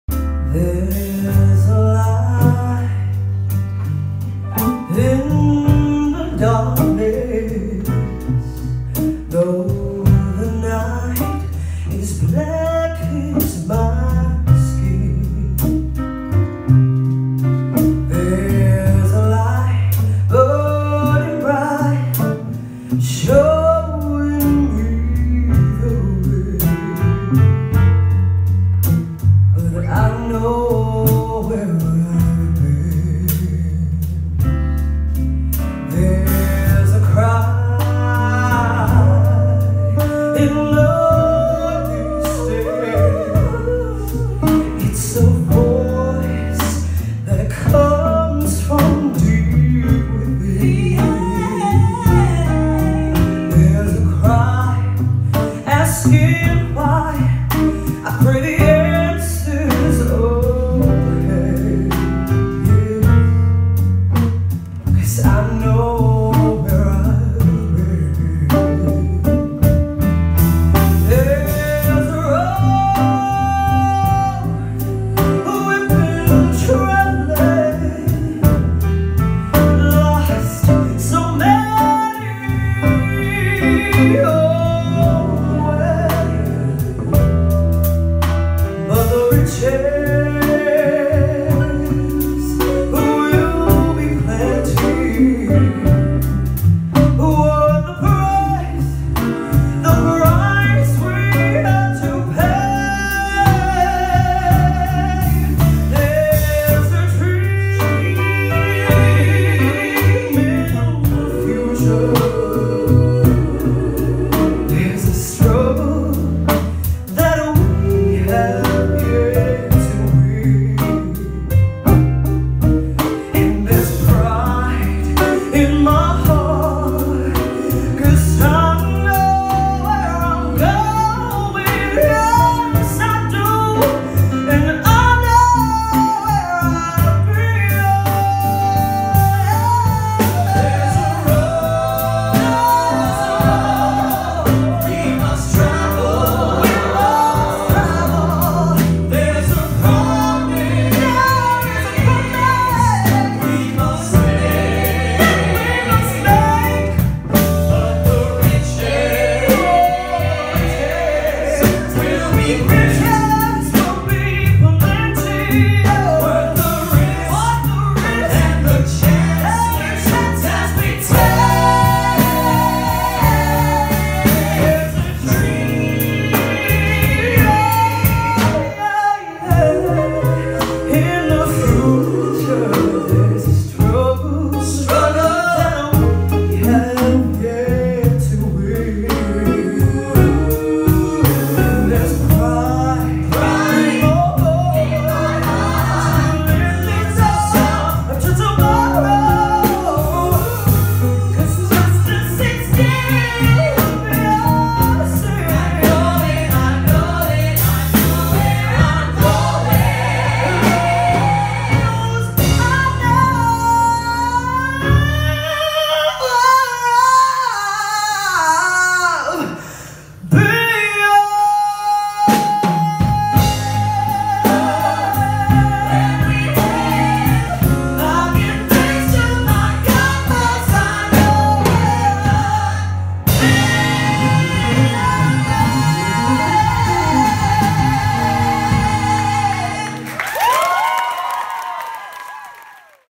DANIELLE STEERS LIVE AT ZEDELS
Danielle Steers performing at Luke Bayer`s debut show live at Zedels, Piccadilly Circus, London